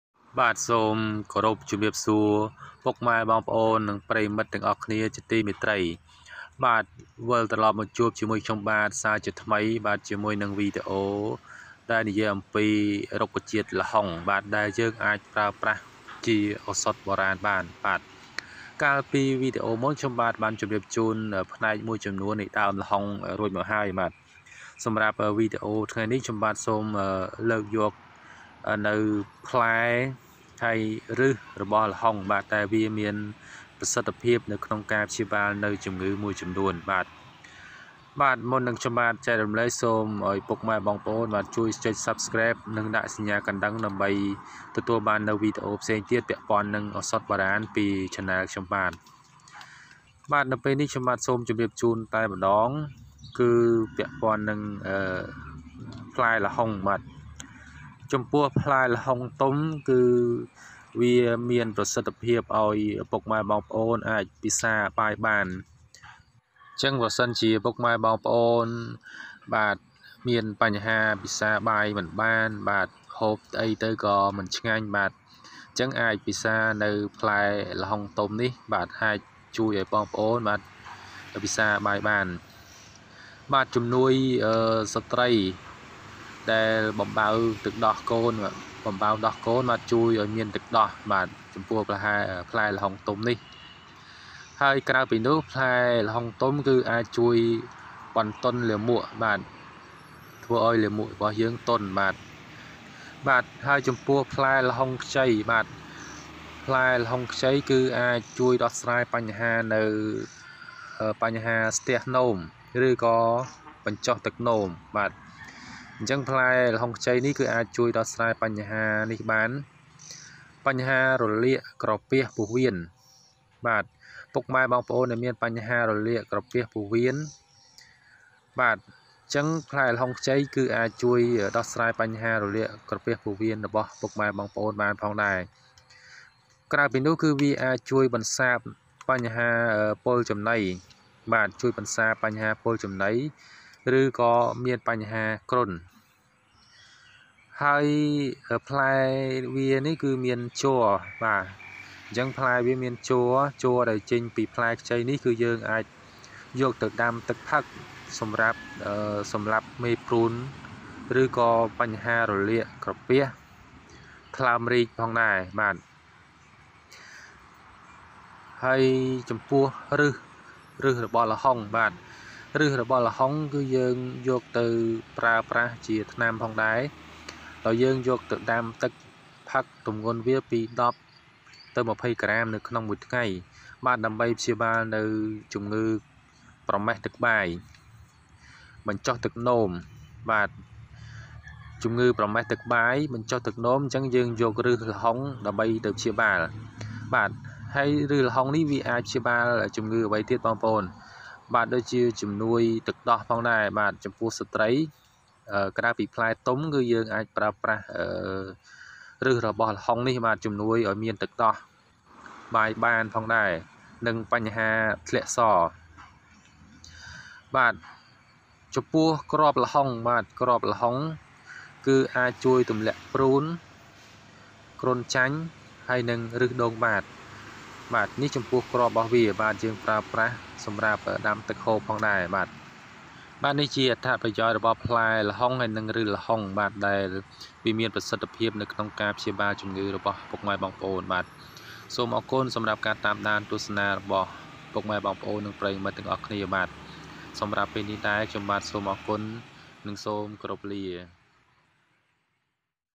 ផ្លែល្ហុង ជាឱសថព្យាបាល តាំងពីឫសដល់ស្លឹក {រុក្ខជាតិឱសថ}
ស្វាគមន៍មកកាន់ឆាណែល រុក្ខជាតិឱសថ​ !!!

ផ្លែល្ហុង ជួយព្យាបាល រលាកក្រពះពោះវៀន ថ្លើមរីក ប្រមេះទឹកបាយ ជំនួយទឹកដោះ {រុក្ខជាតិឱសថ}

សូមអរគុណបងប្អូនអ៊ំពូមីងទាំងអស់គ្នាដែលបានទស្សនាវីដេអូនេះ និងសូមអធ្យាស្រ័យរាល់កំហុសឆ្គងនានាដែលកើតមានឡើងដោយអចេតនា។

សូមអរគុណ!!!